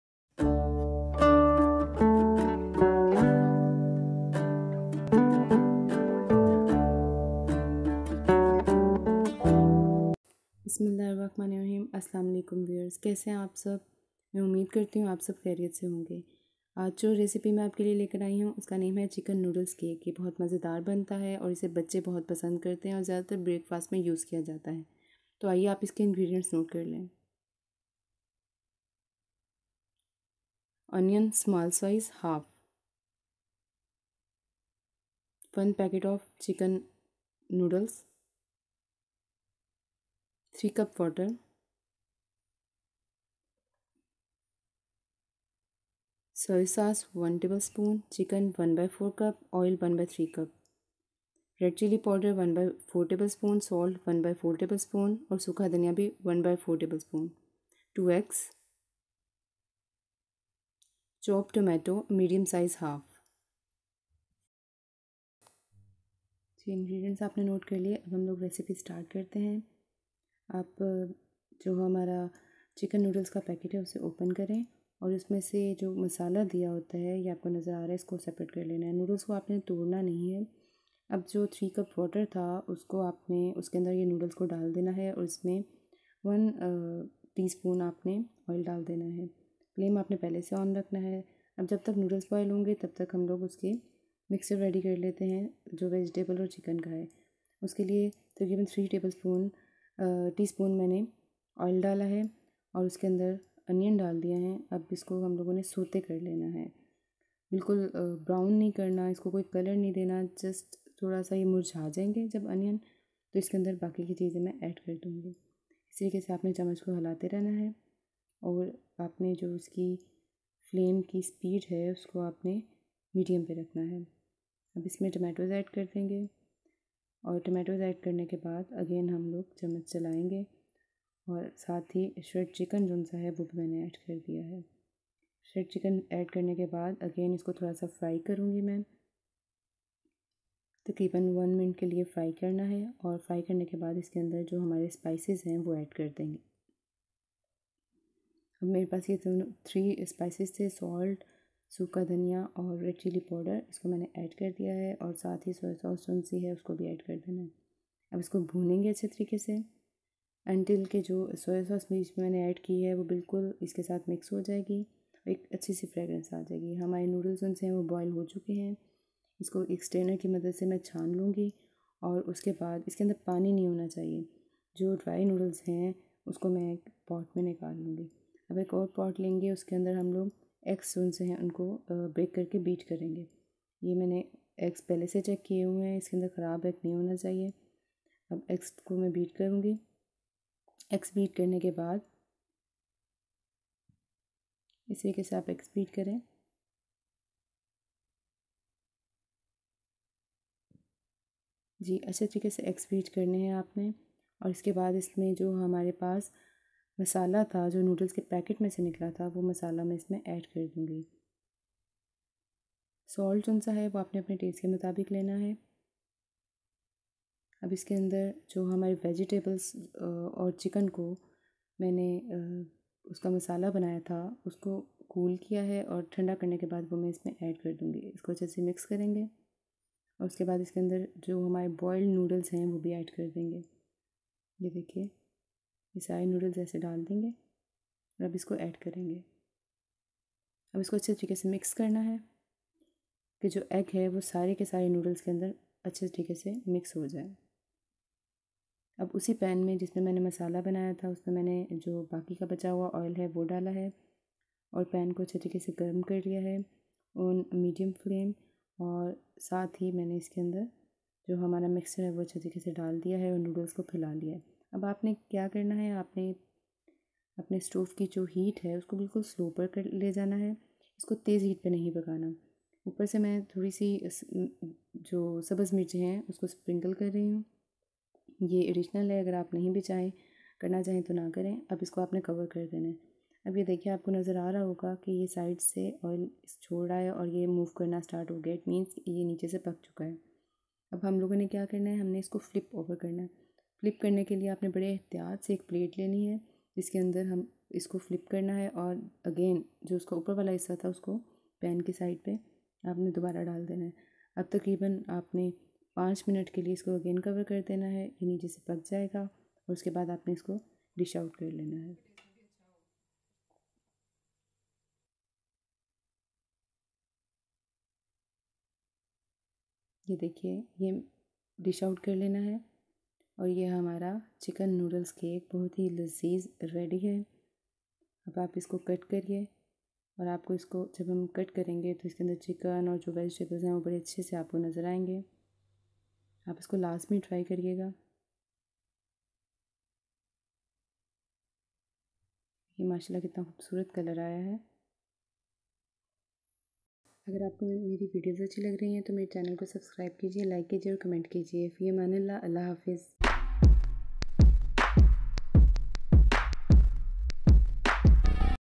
Chicken Noodles Cake | Delicious salty cake | 5 min Recipe in Urdu/Hindi | Maza Zra Hut K
Maza Zra Hut K